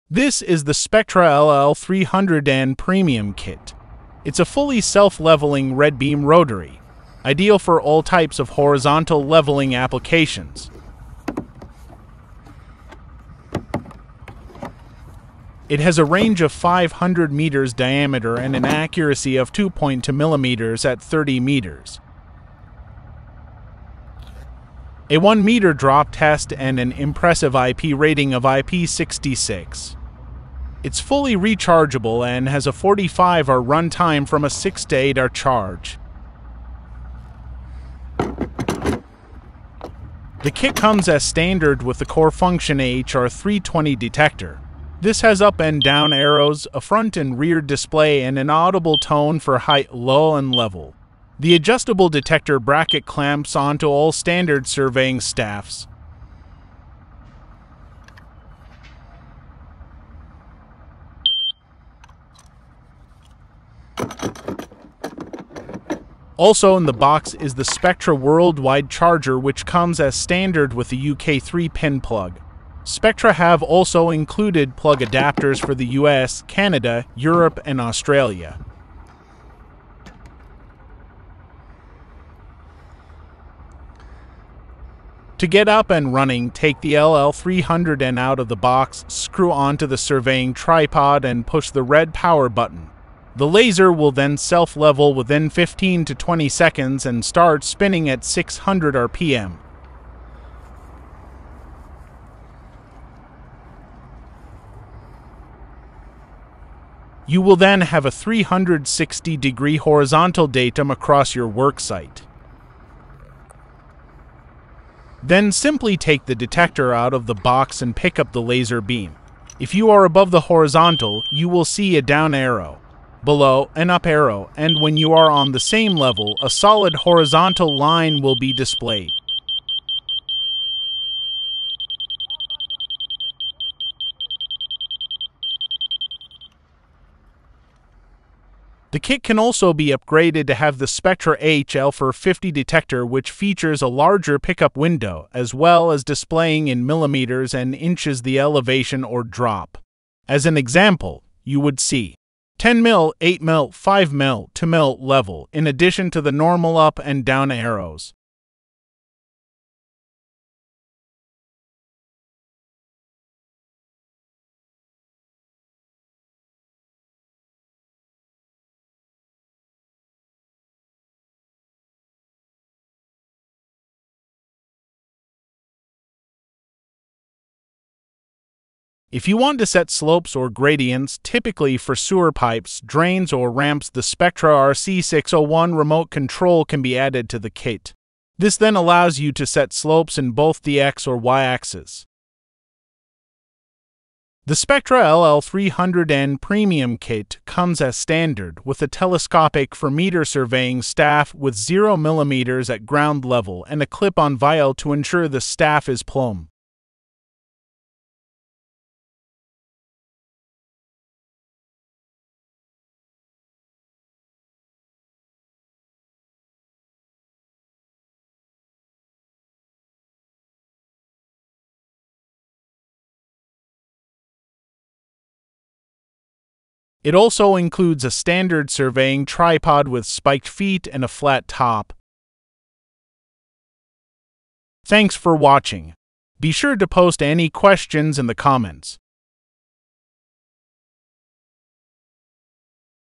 Spectra LL300N Premium Kit - Horizontal Groundwork’s Rotary Laser Level
An overview of the Spectra LL300N heavy-duty ground works rotary laser level kit.

The Spectra LL300N is a premium brand, heavy-duty long-range ground workers laser.

It is a fully self-levelling red beam rotary laser which levels on the horizontal. It is accurate to ±2.2 mm at 30m, range of 500m, has an IP rating of IP66 (Dust tight / Protected against powerful jets of water) and a 6-year warranty from Laser Levels Online. It is rechargeable but can also run on alkaline batteries.